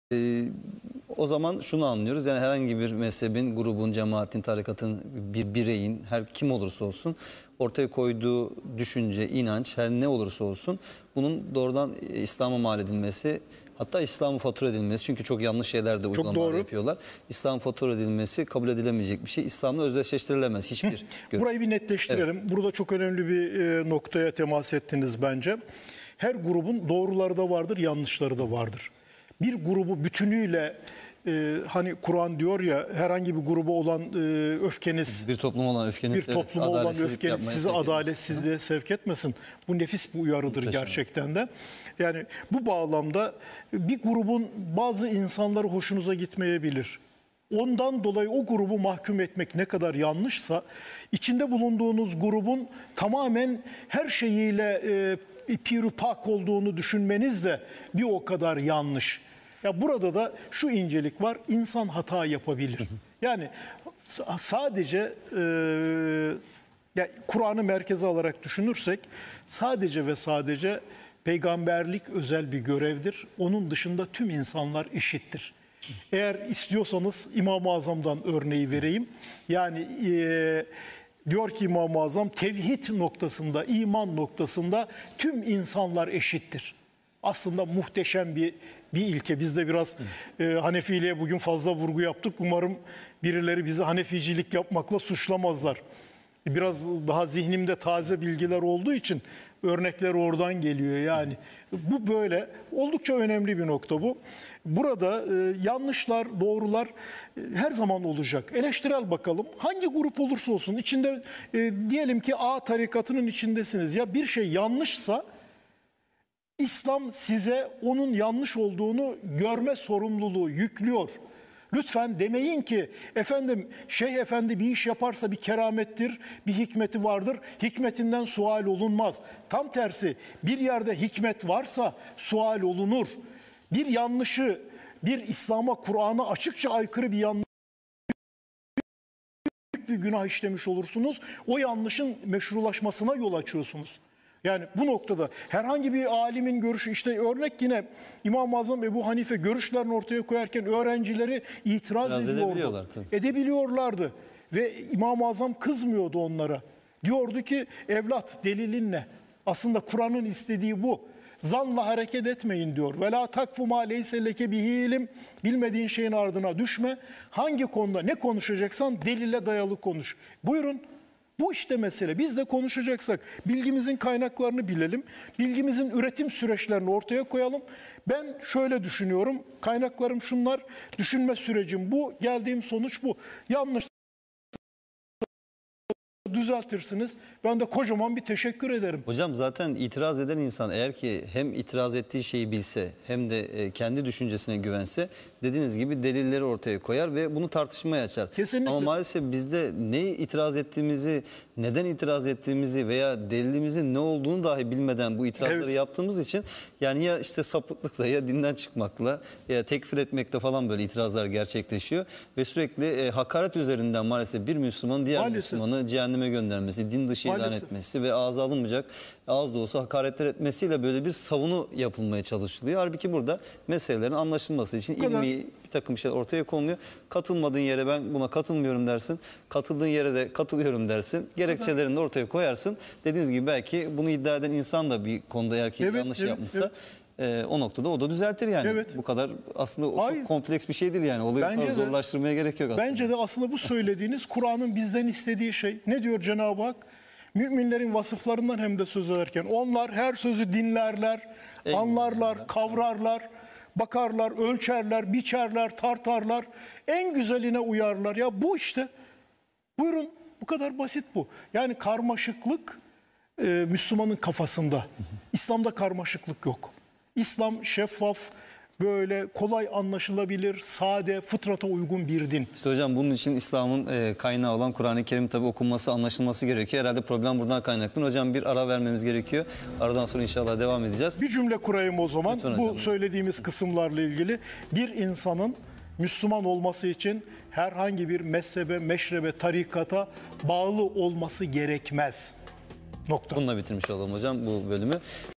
Bir insanın müslüman olması için herhangi bir mezhebe, cemaate bağlı olması gerekmez
Bir insanın müslüman olması için herhangi bir mezhebe, cemaate bağlı olması gerekmez. Her grubun doğruları da yanlışları da vardır. "Tevhid ve iman noktasında tüm insanlar eşittir" der İmamı Azam